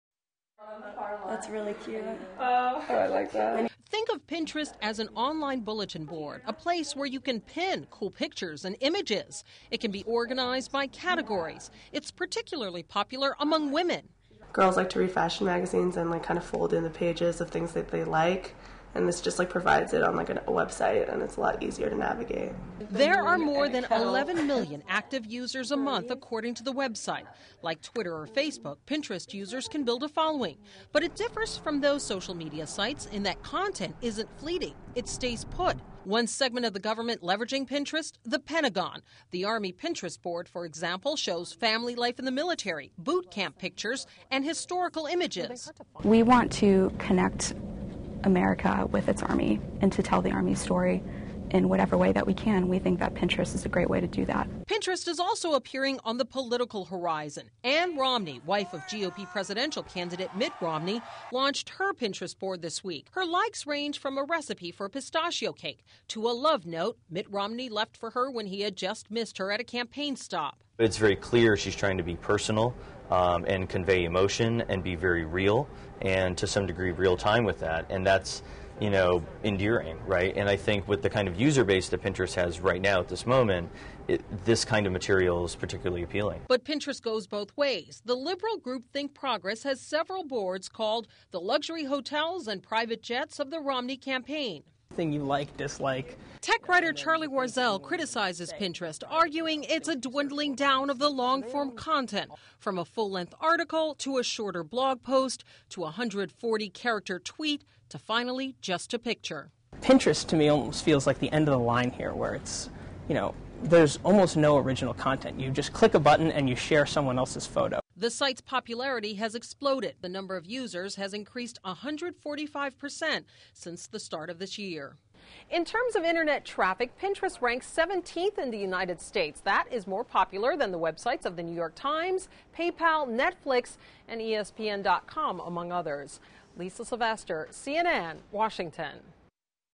The politics of Pinterest
Pinterest is gaining popularity among women, the Army and political campaigns. CNN's Lisa Sylvester reports.